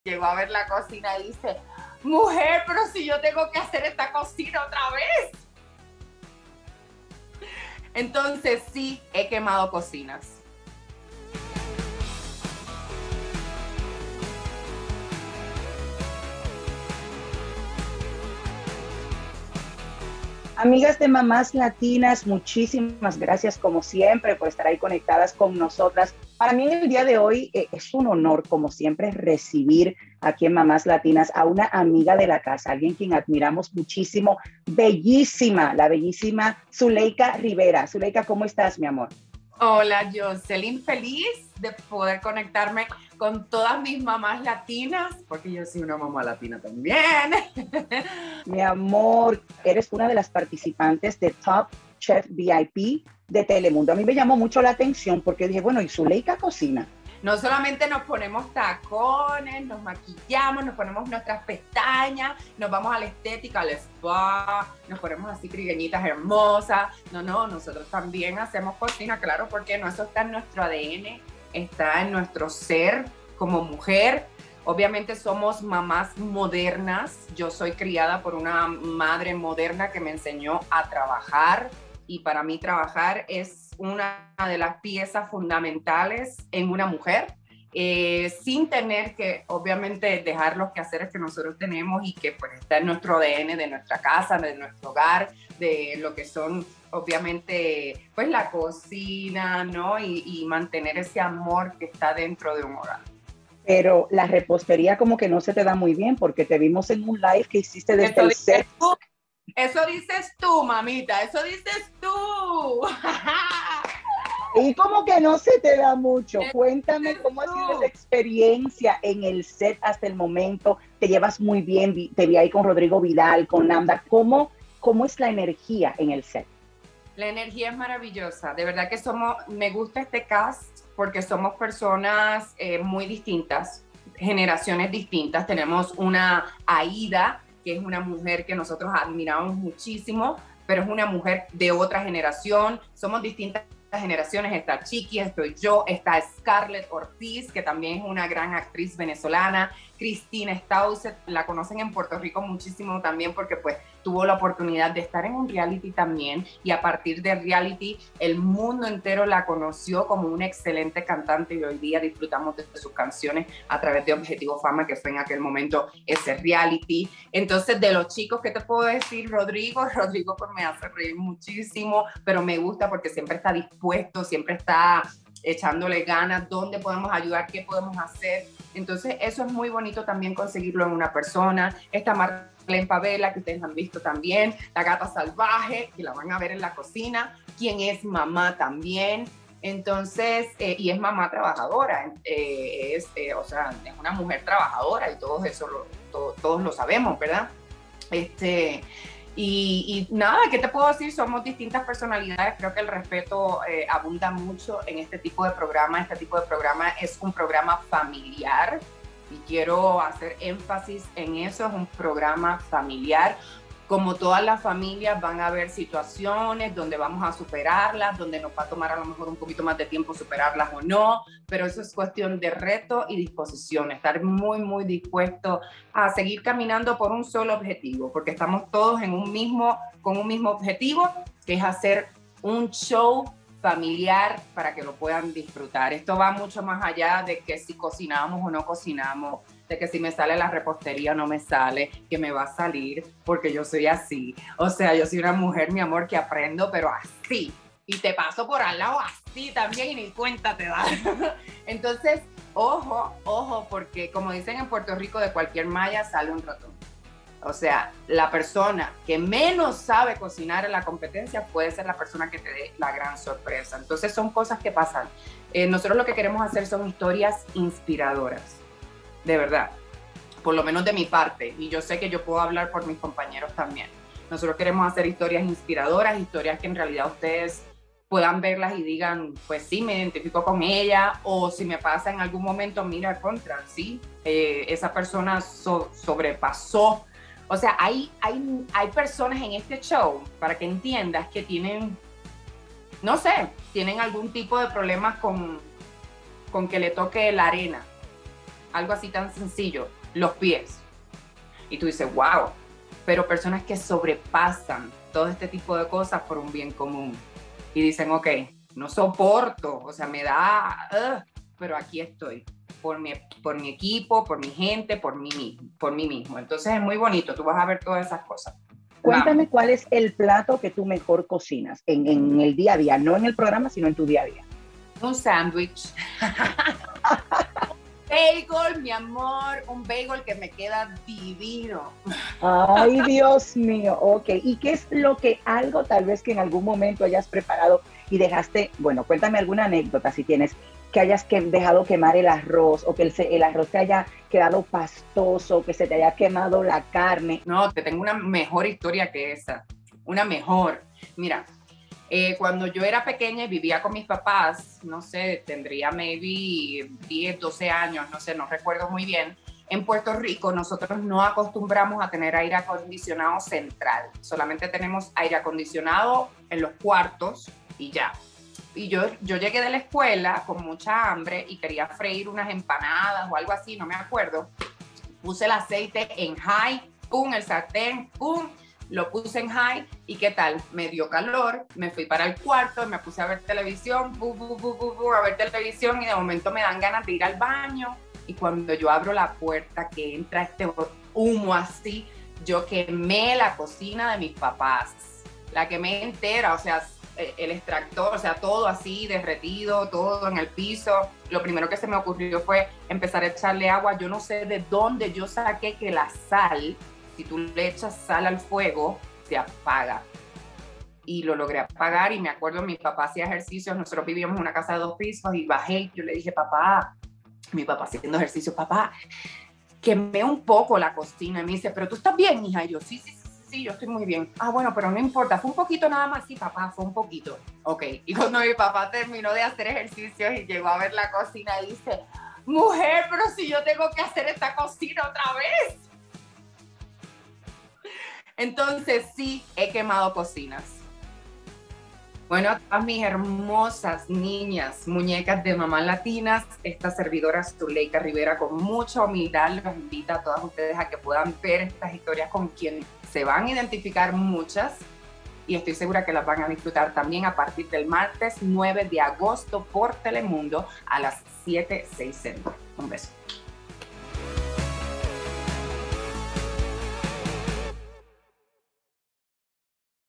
Zuleyka Rivera nos cuenta sobre la vez que quemó su cocina
En esta edición de CelebsYMás tenemos una amiga de la casa, la bella Zuleyka Rivera 💜 quien habló con nuestra Directora Editorial Yoselin Acevedo sobre un incidente que tuvo en la cocina 😱 y reveló uno que otro detalle sobre su próxima participación en el programa ‘Top Chef VIP’ 👩🏾‍🍳